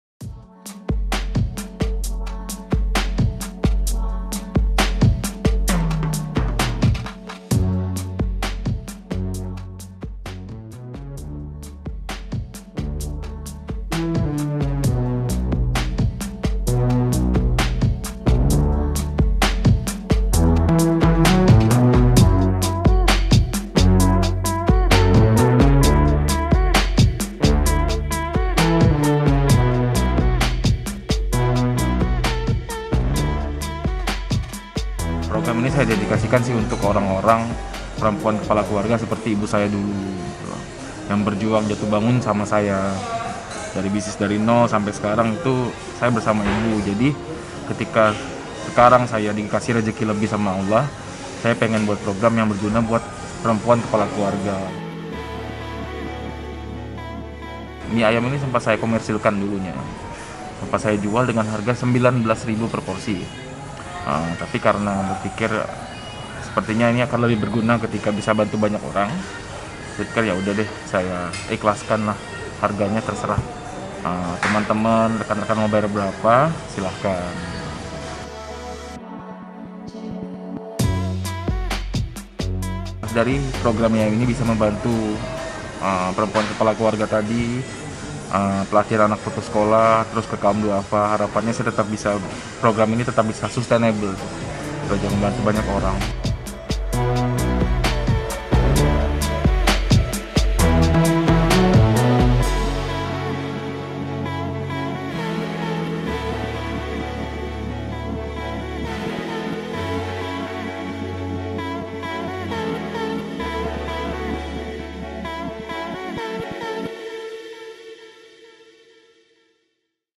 Makan Mie Ayam di Mall, Bayar Seikhlasnya
UMUMNYA, kalau kita membeli makanan di pusat perbelanjaan (mall) cenderung lebih mahal ketimbang di tempat makan luar mal.

Namun, salah satu restoran di dalam mall ini, menyediakan menu mie ayam dengan bayar sesukanya. Syaratnya ikhlas, karena uang untuk membayar mie ayam itu langsung dimasukkan ke dalam kotak amal untuk didonasikan.

Konsep makan mie ayam bayar seikhlasnya ini merupakan program donasi Mie Ayam Marta yang berlokasi di mall Lotte Mart Fatmawati, kawasan Cipete, Jakarta Selatan.

Program donasi restoran yang buka mulai pukul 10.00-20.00 WIB ini, sudah dimulai sejak 13 Juli 2020. Saat ini rata-rata menyiapkan hampir 100 porsi mie ayam per hari.

Ide berawal ketika pemilik restoran Restu Zulfikar merasa prihatin, saat kedatangan seorang konsumen yang mau makan, namun uangnya tidak cukup.

Donasi yang terkumpul akan digunakan untuk membantu warga yang membutuhkan dan perempuan kepala keluarga.

''Dulunya, saya juga merintis usaha restoran ini dari nol, jatuh bangun berdua bersama ibu,'' ujar pria kelahiran Pontianak ini.